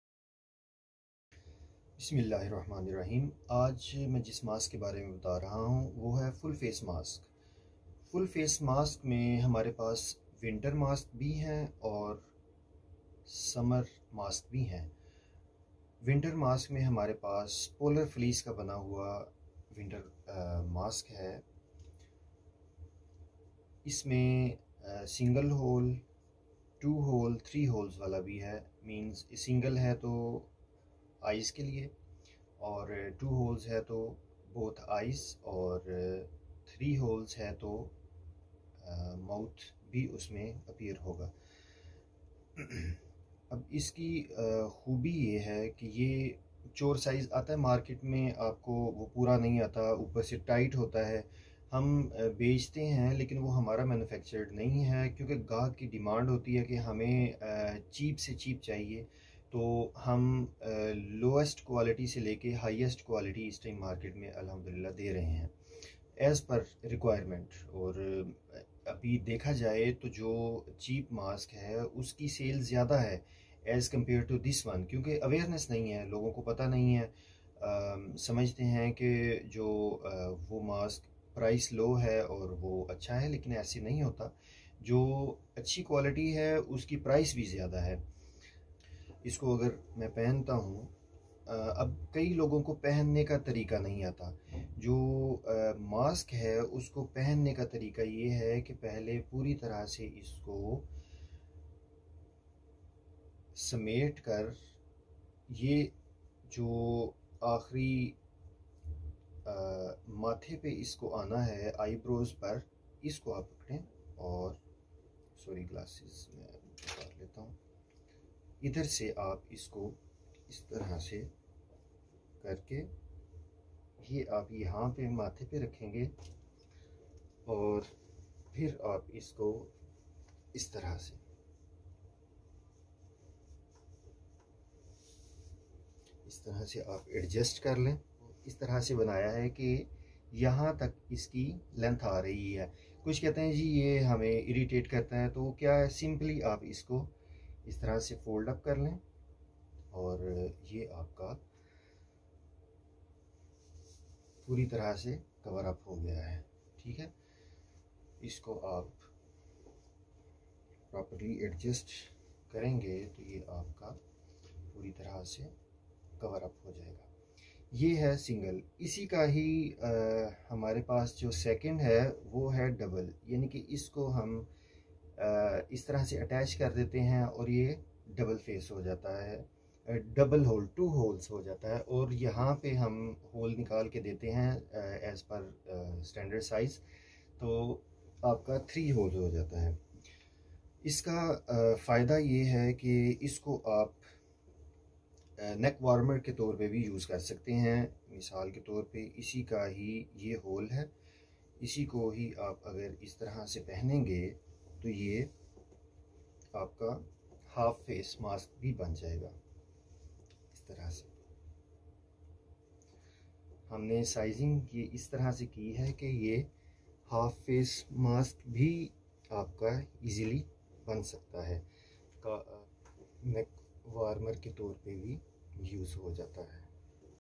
MSB Balaclava Full Face Fleece Mask Thermal Winter Reusable Hood Cap Cover Different colors Mask
Condition: Brand New
Material: Polar Fleece - Double Raised
Height:38cm
Back Height:38cm
Head Circumference:60cm
Weight 65g
Quantity: One Piece
Please Note: As we all know, the different computers display colors differently, so the color of the actual item may vary slightly from the following images.